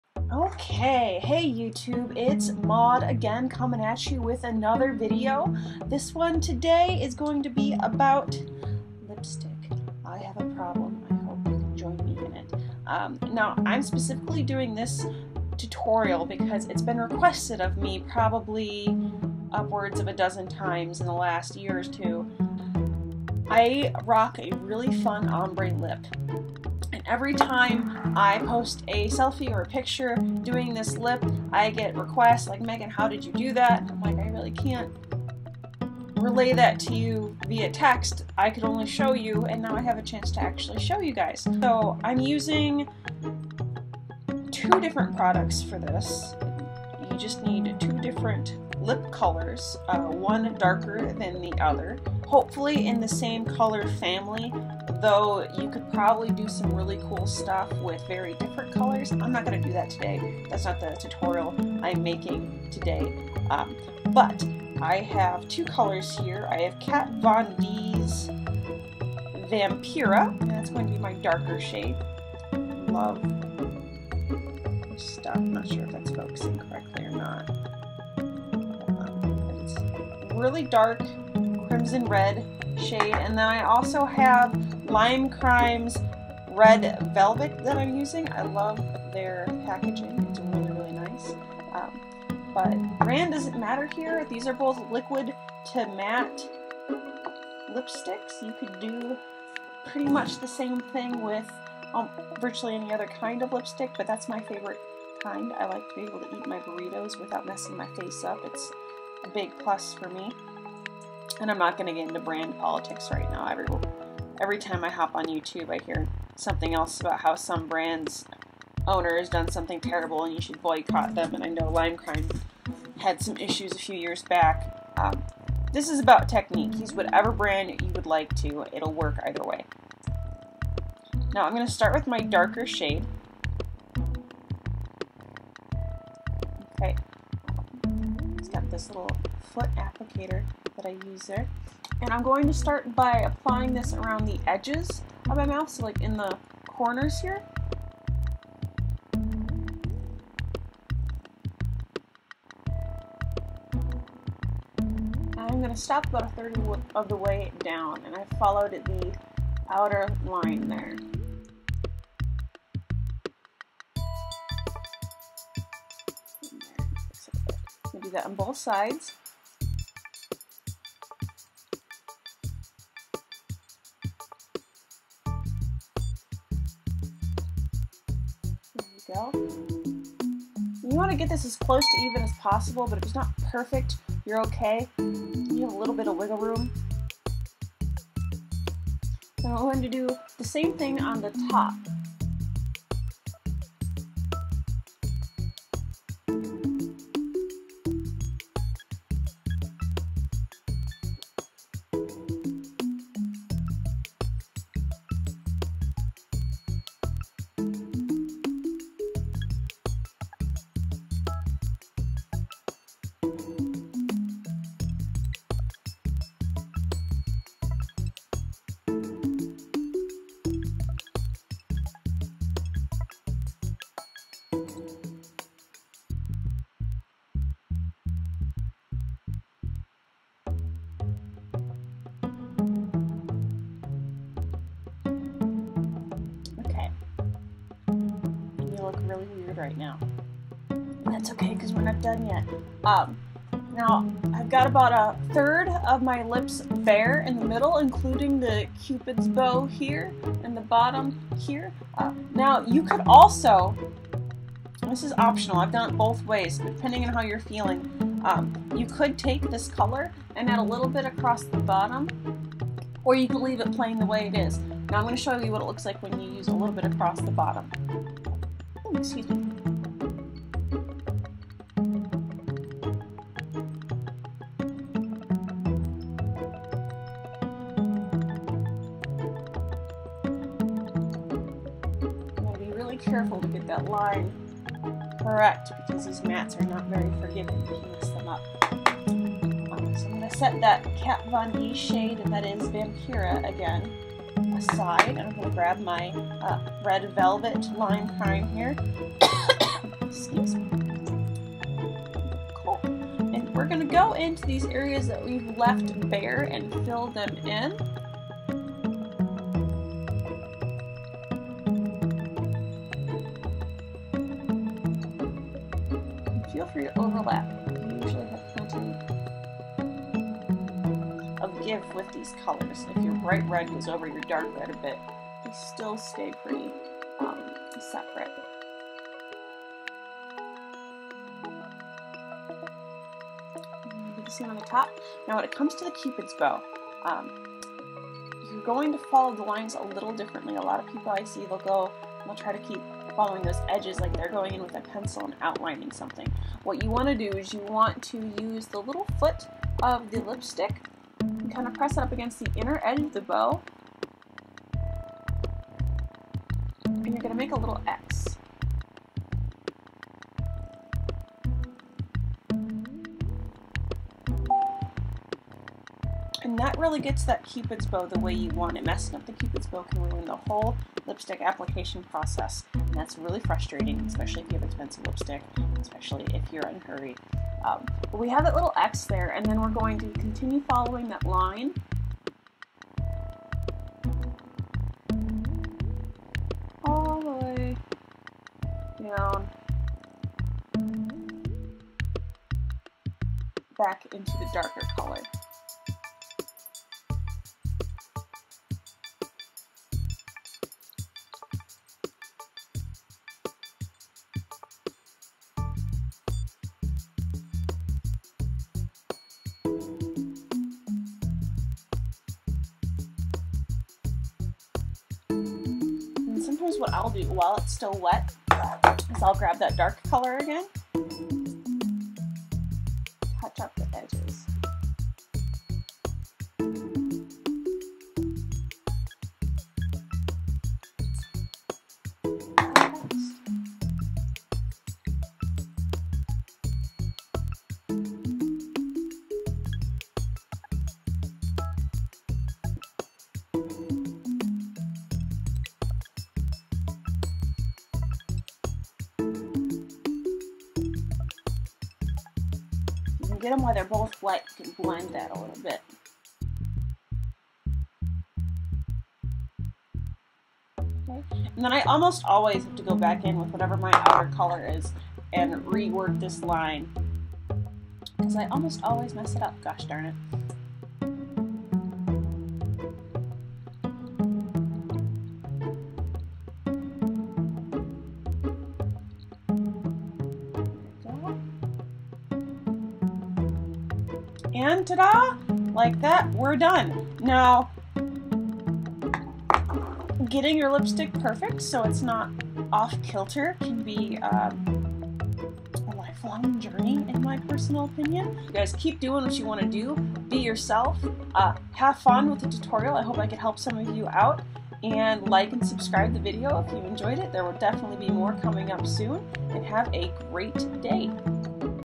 Red Lip Ombre Tutorial
**Edit** Yeah, my top lip was hella uneven, hopefully that wasn't too distracting! **

A simple crimson to bright red lip ombre!